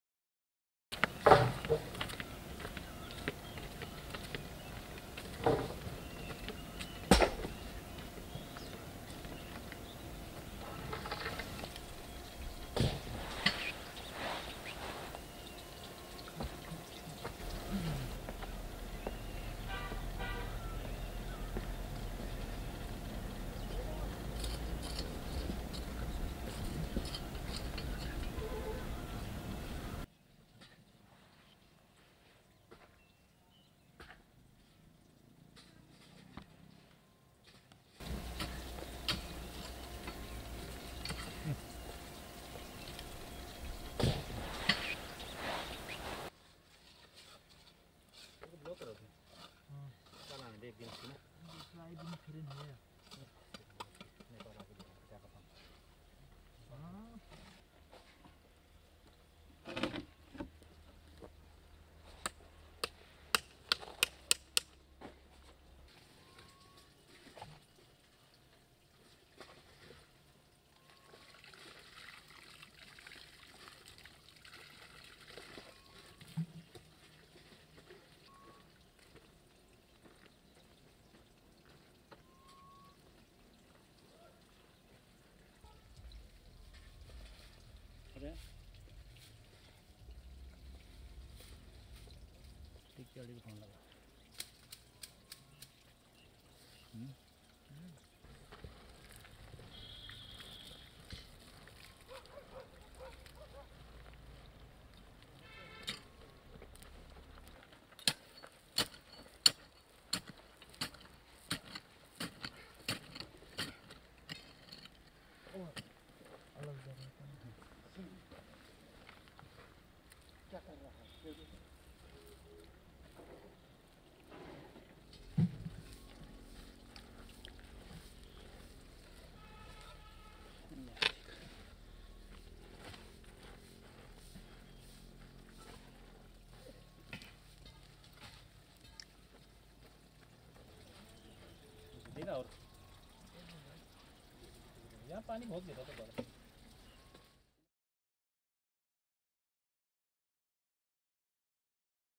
Planting grafted Cherry trees from Kashmir at wildfilmsindia botanical collections at Jabbarkhet
In the misty late winter weather, bare root fruit tree saplings are planted, as our malis do the thala dance after placing the roots and stems into the ground. The wildfilmsindia team plants grafted hybrid two year old cherry saplings sourced from Kashmir at the wildfilmsindia botanical collections at Jabbarkhet in Uttarakhand.